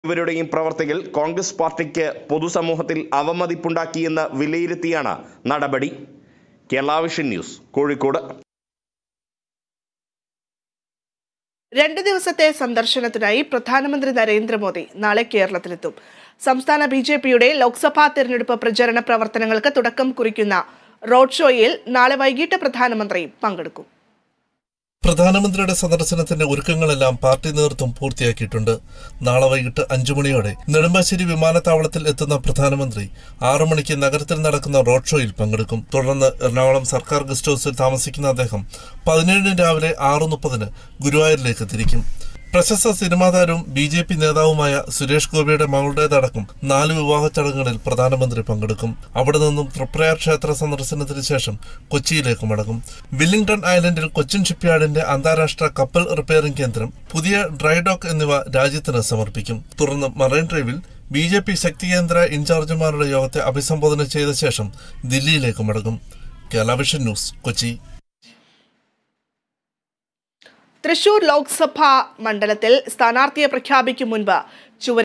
പ്രധാനമന്ത്രി നരേന്ദ്രമോദി നാളെ കേരളത്തിലെത്തും; റോഡ് ഷോയില്‍ നാളെ വൈകിട്ട് പങ്കെടുക്കും
About COA